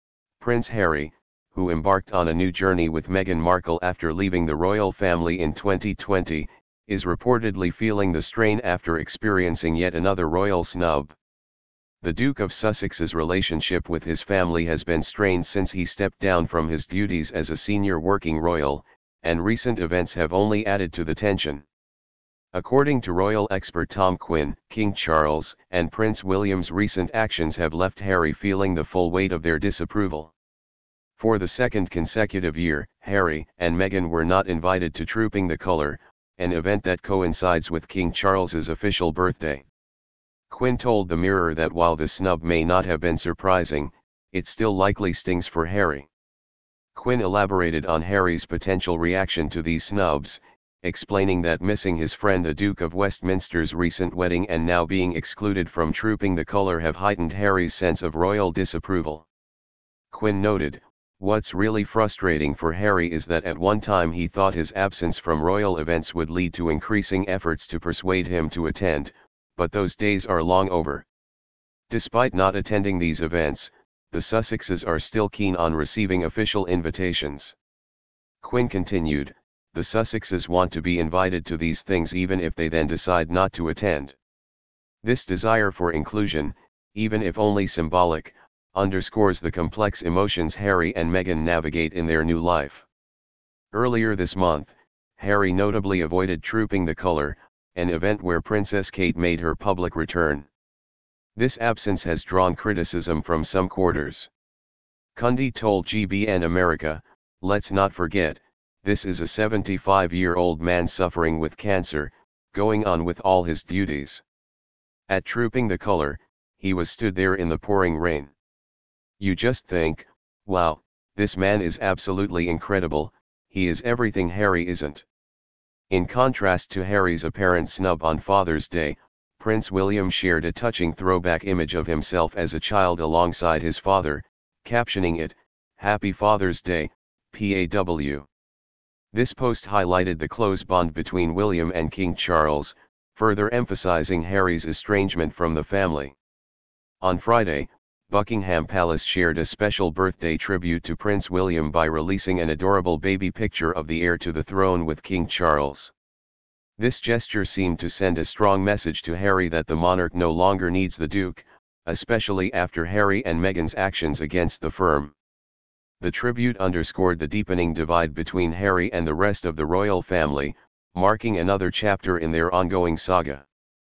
Prince Harry left 'hurt' by King Charles latest decision
Explore the fallout from King Charles' recent decision that has reportedly left Prince Harry feeling hurt and excluded. We analyze the implications for the royal family dynamics and the ongoing tensions between Harry and the monarchy.